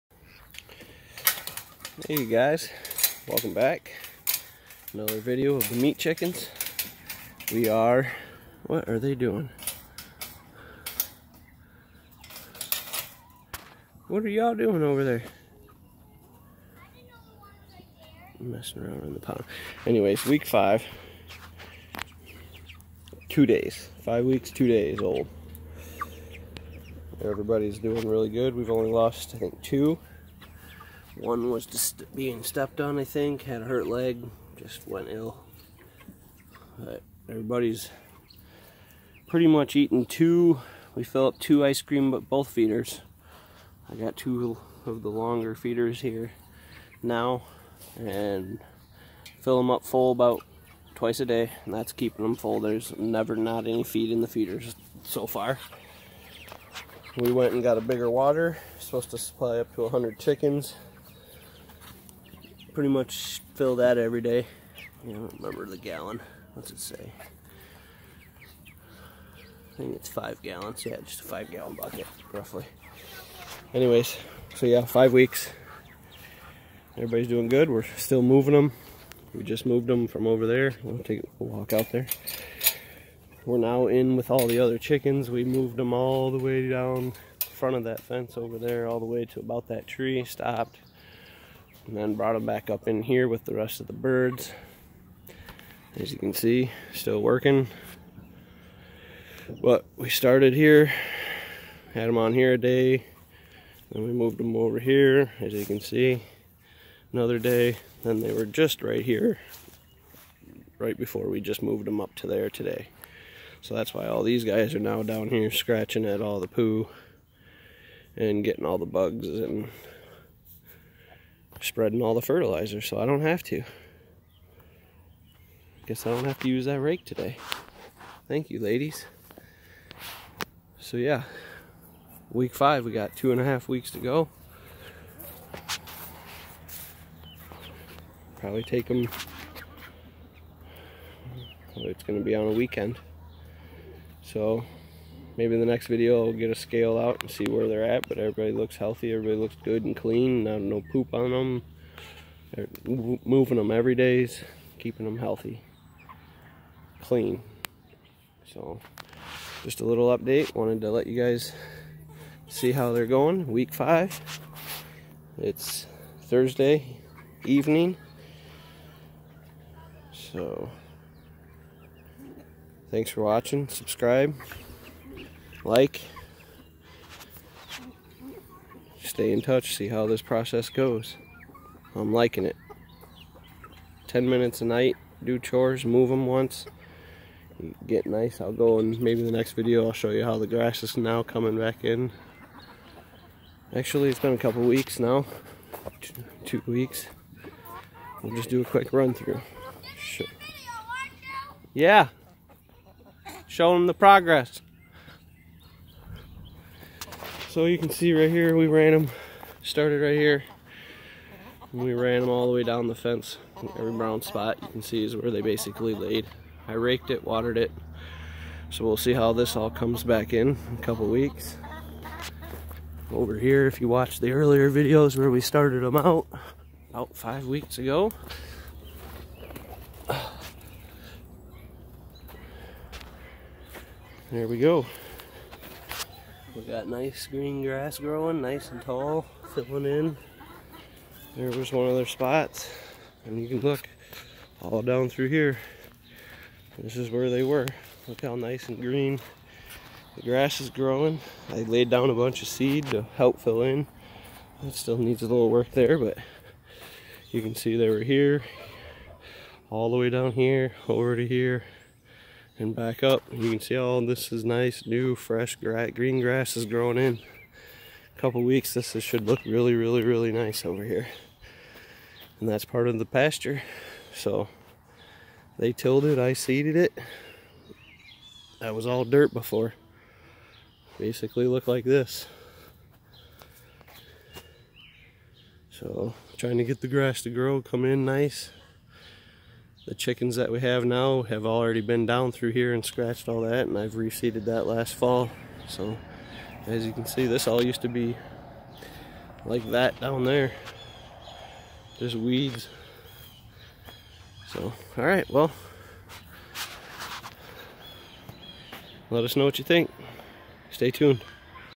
Raising Meat Chickens Right!! WEEK 5 UPDATE!!
Week 5 Update on raising our meat birds here on O Valley Farms